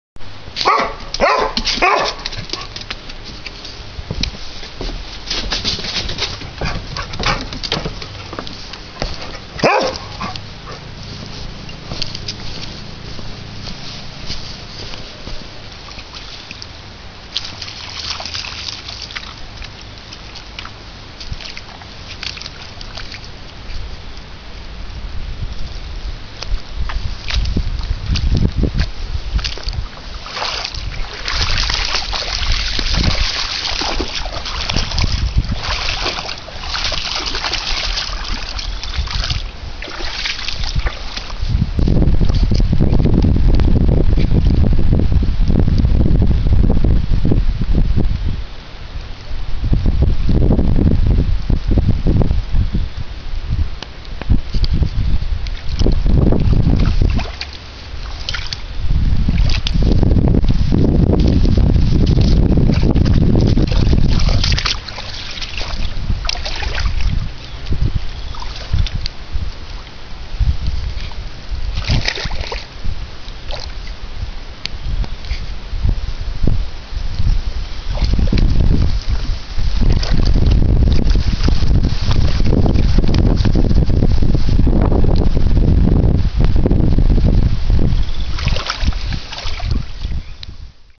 Scout and the Flood - spring 2011
A new threat looms where gophers once roamed.  Scout investigates.